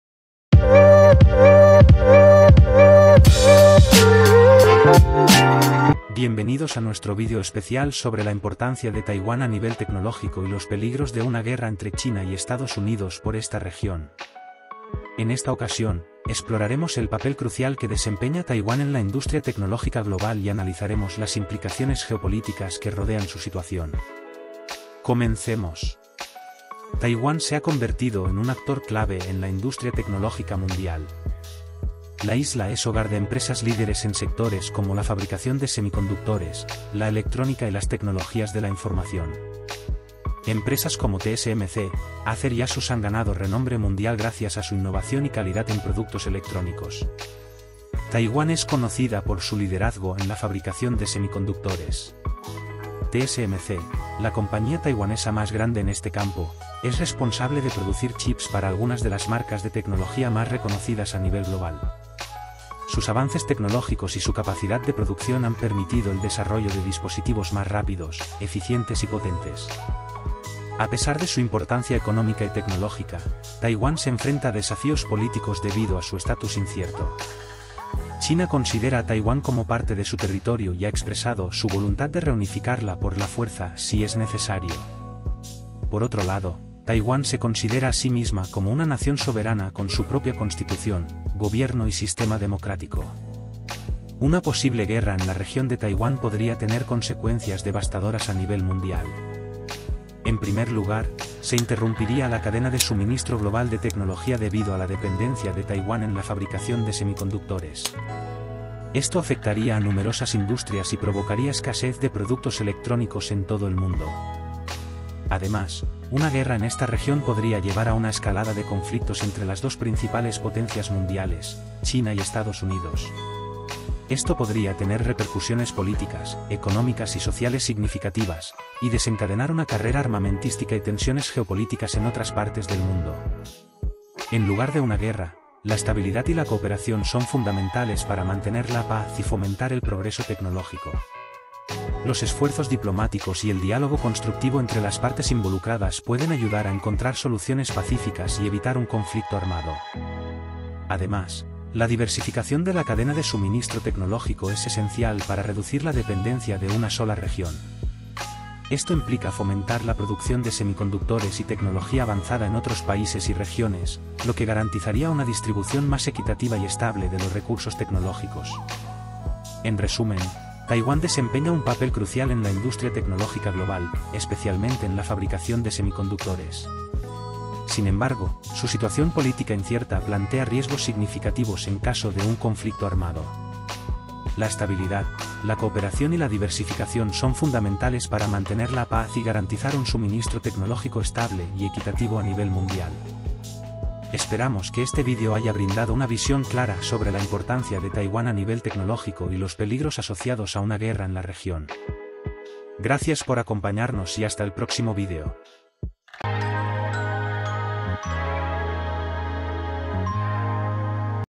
TODO lo que tienes que saber de Taiwan en 3 minutos
En este video, descubre la importancia de Taiwán en la industria tecnológica global y explora los riesgos geopolíticos asociados a su situación. Analizamos el liderazgo de Taiwán en la fabricación de semiconductores, su papel en la cadena de suministro tecnológico y las implicaciones de una posible guerra en la región. Acompáñanos en este viaje informativo sobre la importancia de Taiwán y los desafíos que enfrenta en el panorama tecnológico global.